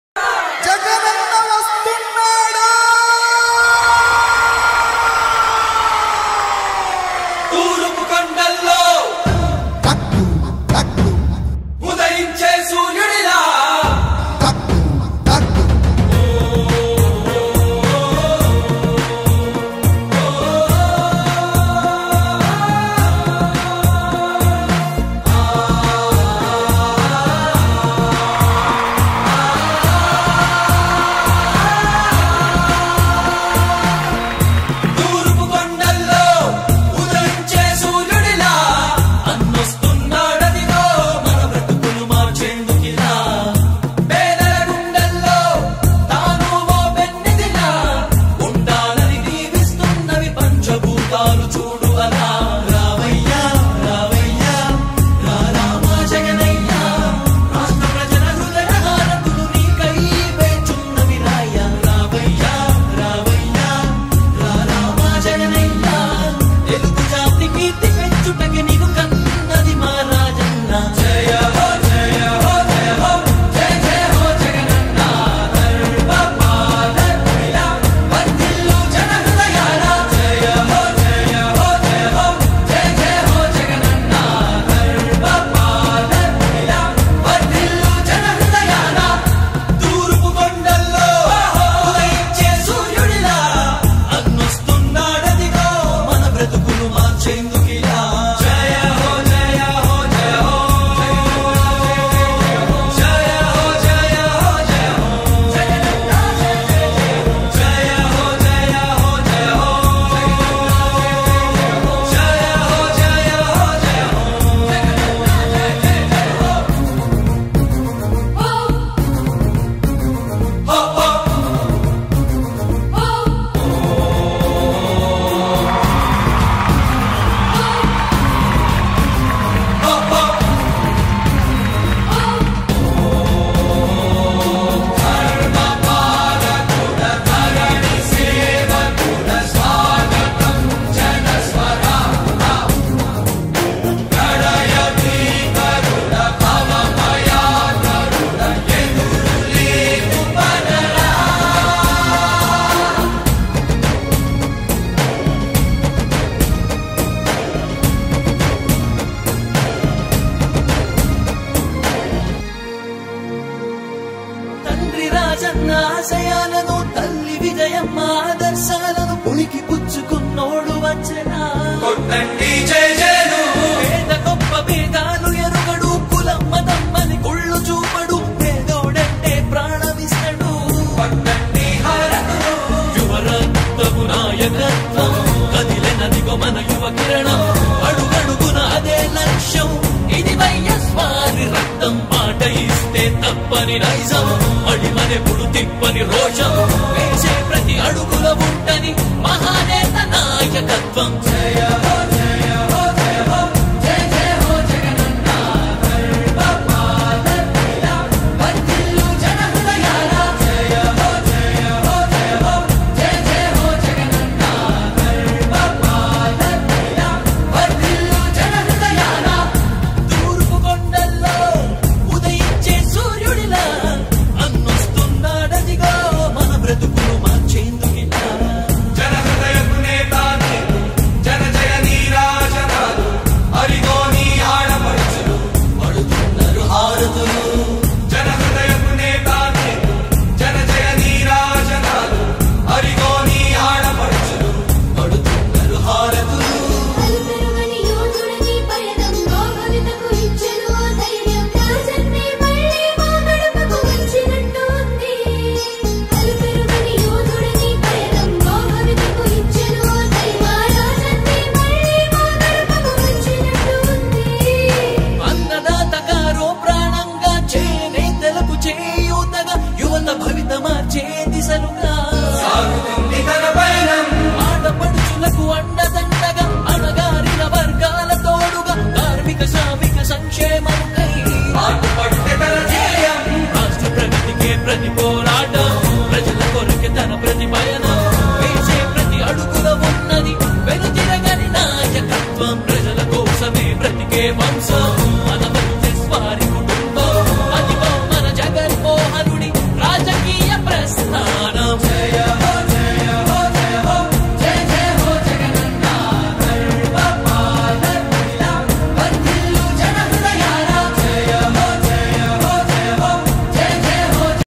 Must Watch: చంద్రబాబు సొంతగడ్డపై పులిలా అడుగుపెట్టిన జగన్ |YS Jagan Goosebumps Entry |RP
Watch :